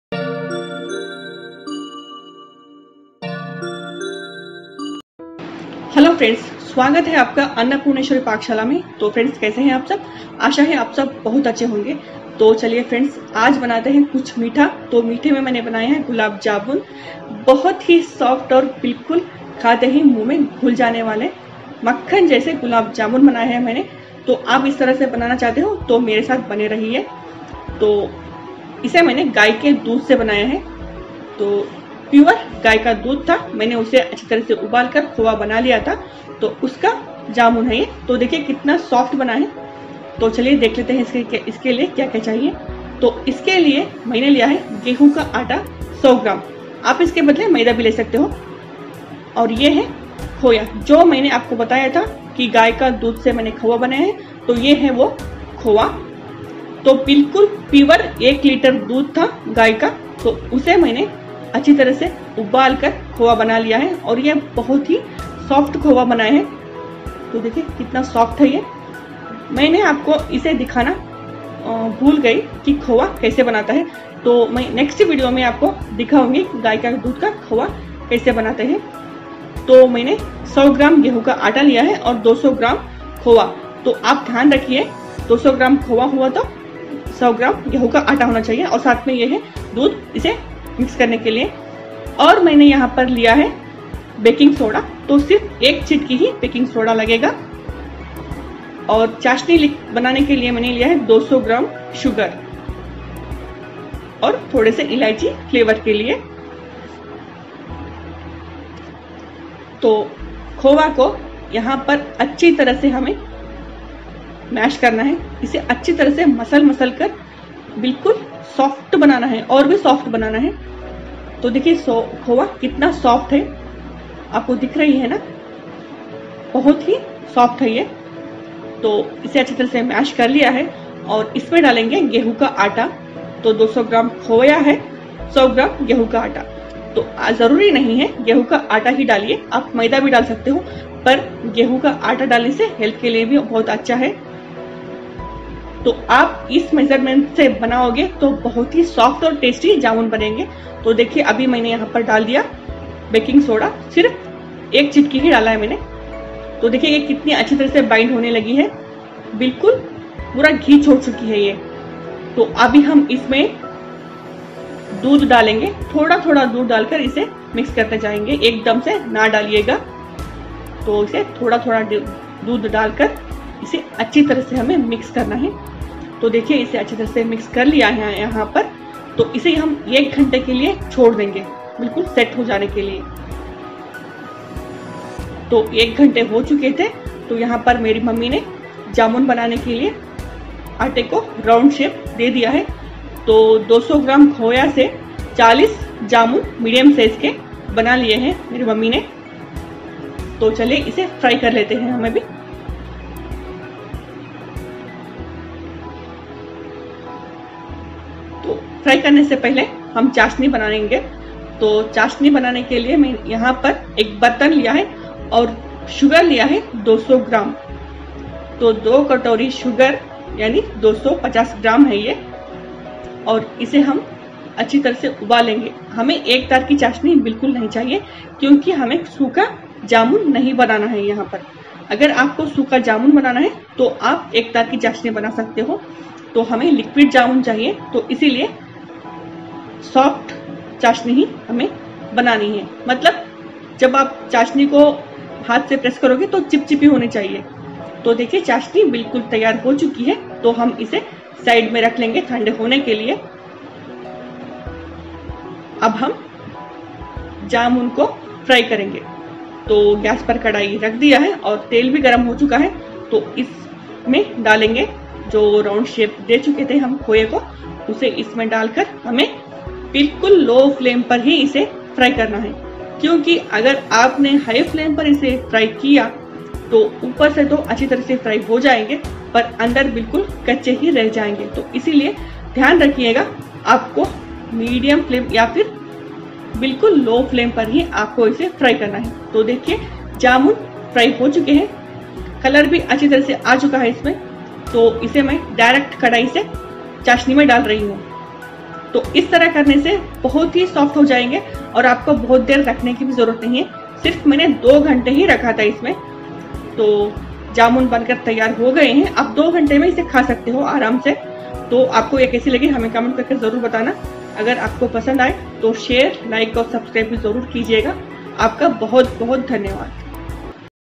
Khoya Gulab Jamun Halwai Style Me || Soft Aur Tasty// Gulab Jamun 🤗
Hello Friends

Maine Banaya Hai Bilkul Soft Ek Dum Ghuljaane Vaale Gulab Jamun, Isse Maine Hai Ke Doodh Se Banaya Hai  Maine Hai Ke Ek Ltr Hai Ke Doodh Se Khoya Banaya Aur USS Se Jamun Banaye, Sach Me E Jamun Bahut Hi Tasty Aur Bahut Soft Band Hai, Aap isse Jarur Try Kariye Aur Mujhe Aapke Anubhv Bataiye Dhanyvad